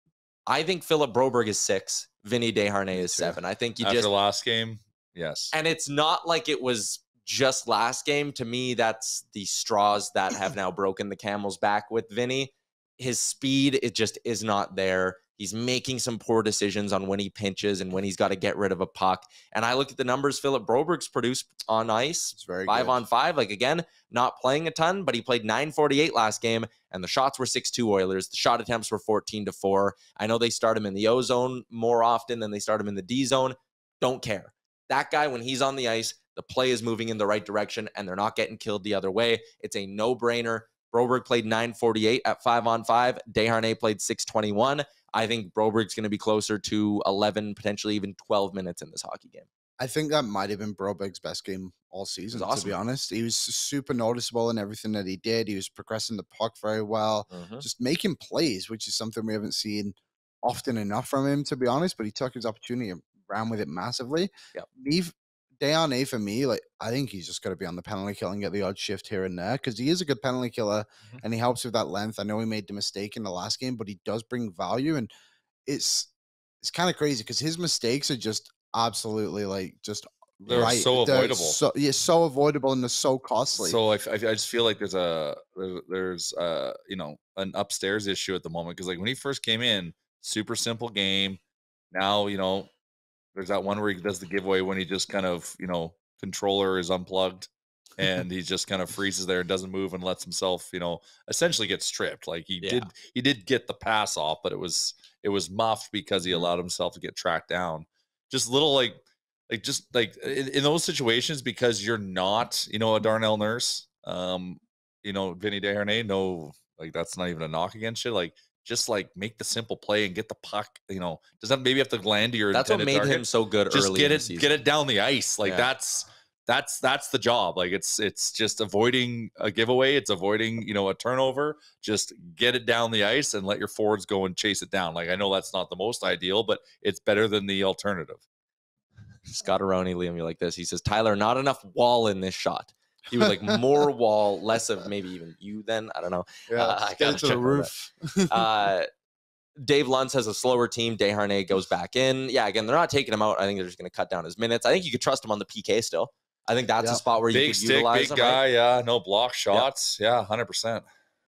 Philip Broberg has turned into a nice surprise the Oilers
Philip Broberg has turned into a nice surprise the Edmonton Oilers